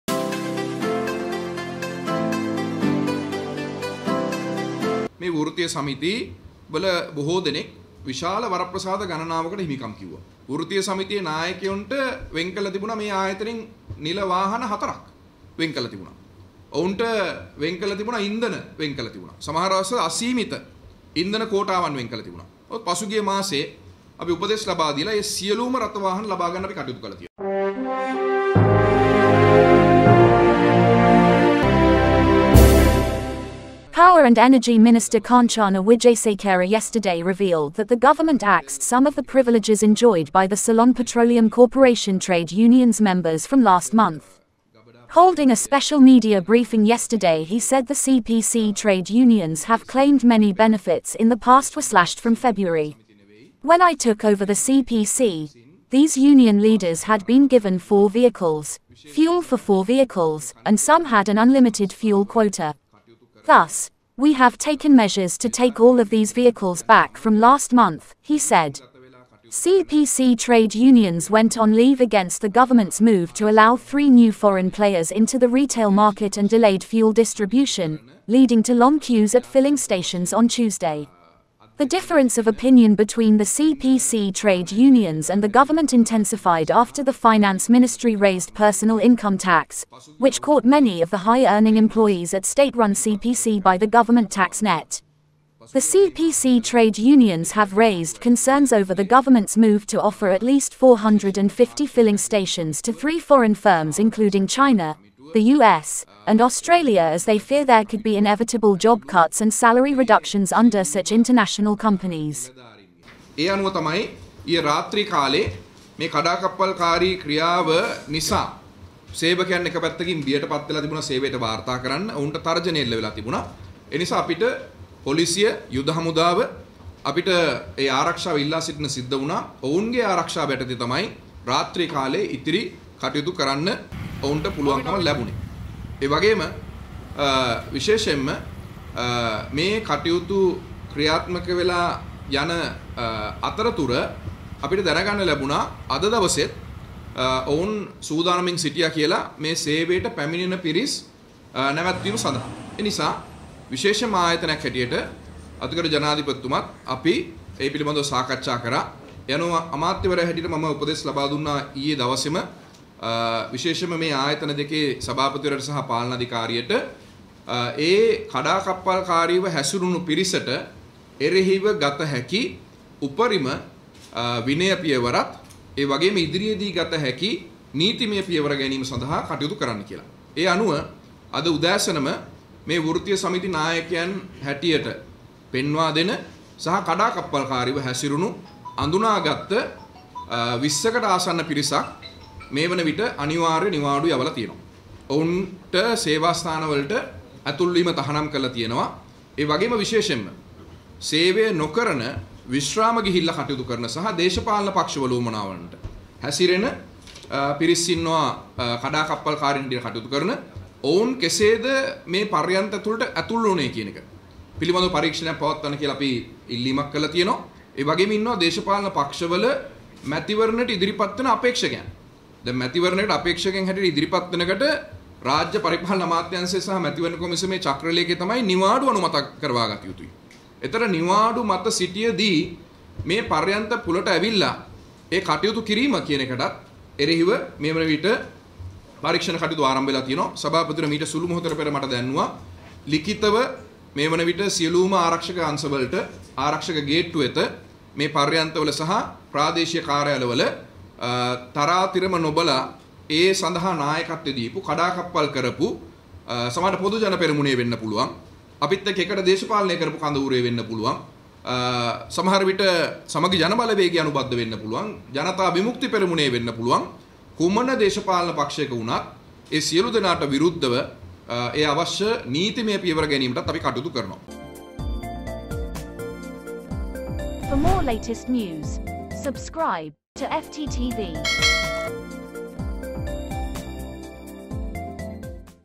Govt. axes CPC trade union privileges: Kanchana
Power and Energy Minister Kanchana Wijesekera yesterday revealed that the Government axed some of the privileges enjoyed by the Ceylon Petroleum Corporation (CPC) trade unions members from last month.